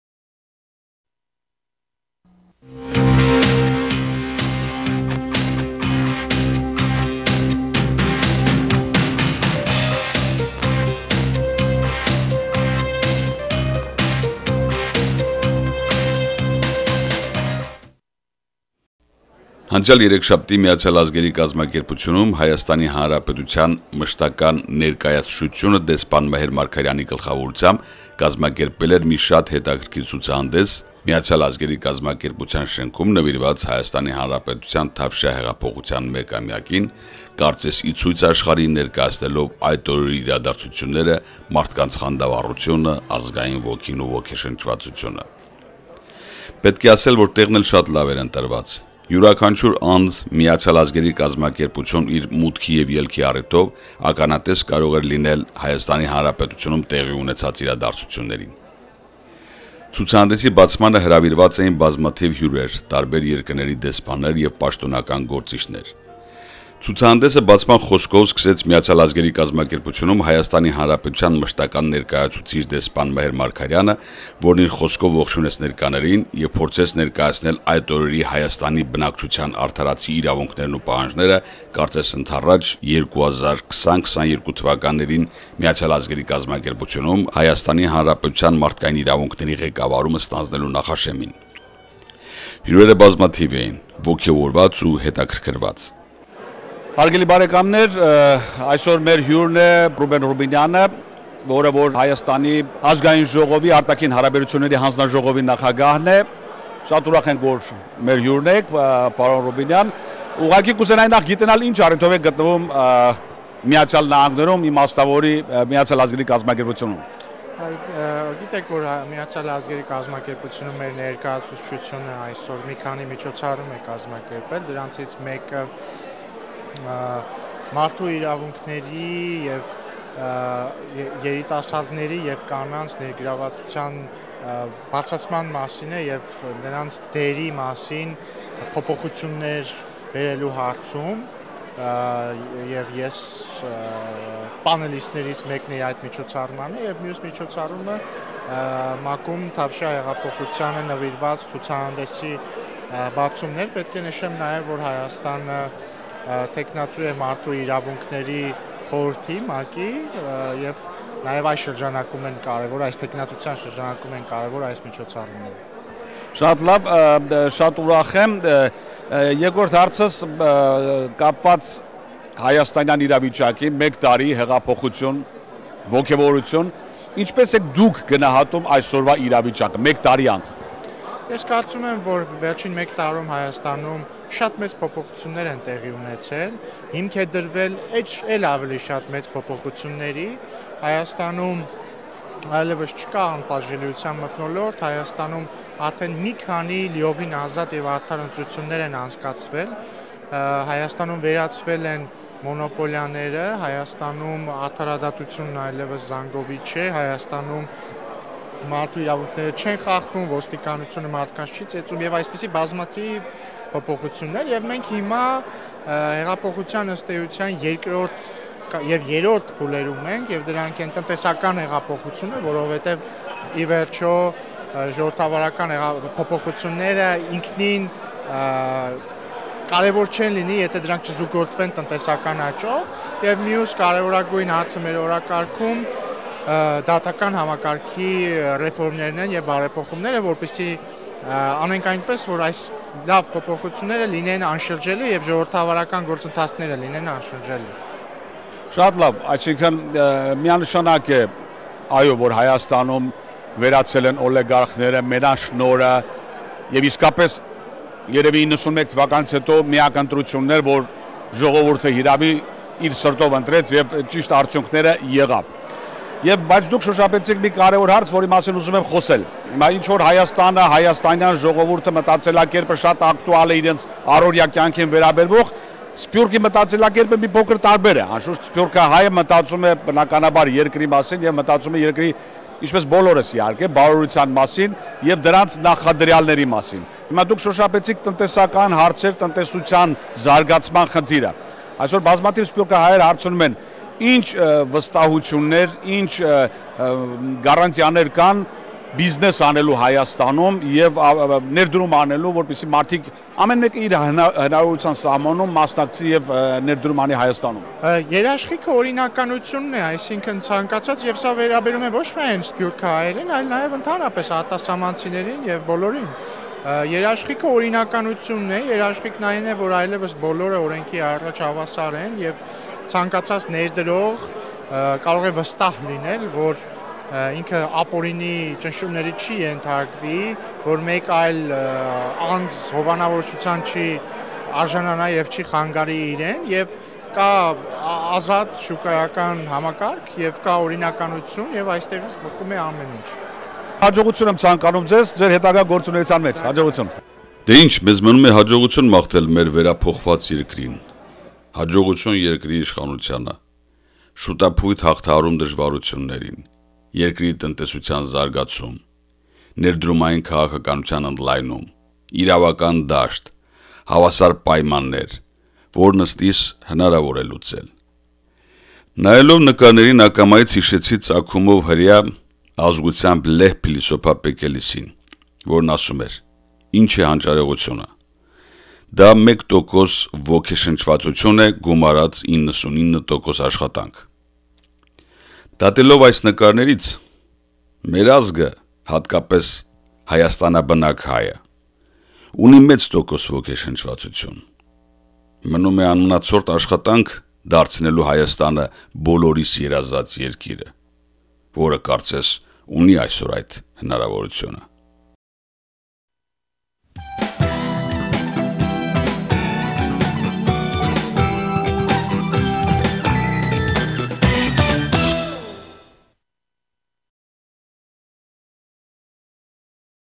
Ardzagang Armenian TV presents: Armenian Exhibition in the United Nations, New York.
Ardzagang Armenian TV presents
Armenian Exhibition in UN, New York.
Dedicated to the 1st Anniversary of Velvet Revolution in Republic of Armenia.
Attended by a large number of ambassadors and delegates of the UN member states, representatives of the UN Secretariat, as well as members of the Armenian community, the event featured remarks by the Permanent Representative of Armenia to the UN Mher Margaryan.
Ruben Rubinyan, Chair of the Standing Committee on Foreign Relations of the National Assembly of Armenia, briefed the guests on the evolution of the events depicted in the exhibit as well as on the parliamentary activities in the field of foreign relations.